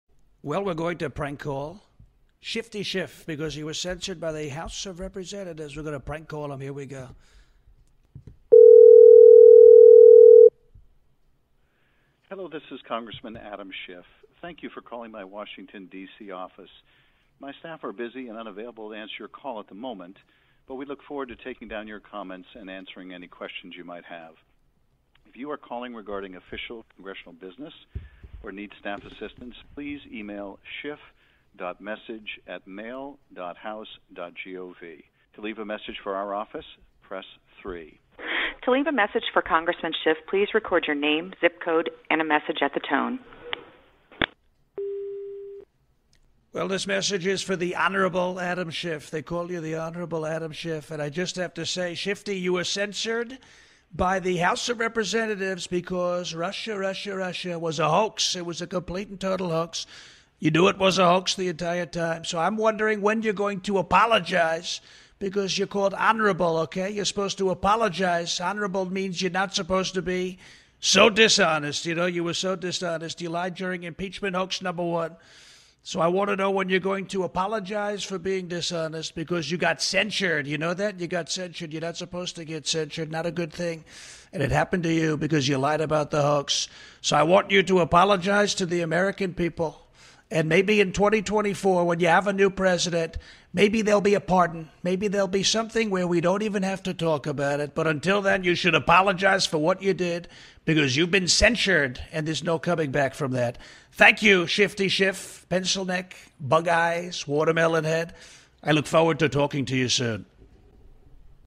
"TRUMP" LEAVES ADAM SCHIFF A VOICEMAIL!
I left Adam Schiff a big beautiful voicemail today the only way I know how.